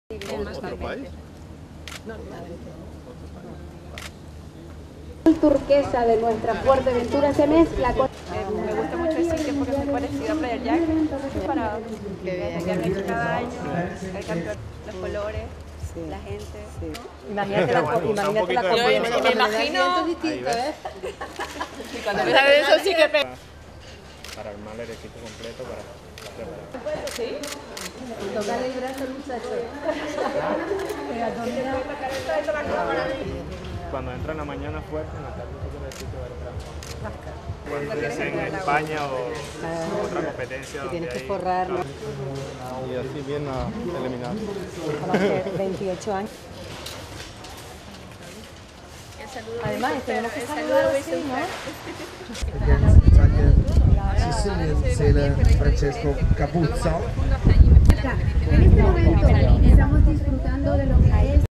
Fuerteventura World Cup 2013.- Visita de la secretaria de Estado de Turismo y delegada del Gobierno
La secretaria de Estado de Turismo, Isabel María Borrego Cortés, acompañada por la delegada del Gobierno en Canarias, María del Carmen Hernández Bento, visitó hoy la isla de Fuerteventura y asistió al Campeonato Mundial de Windsurf y Kiteboard en Playa de la Barca.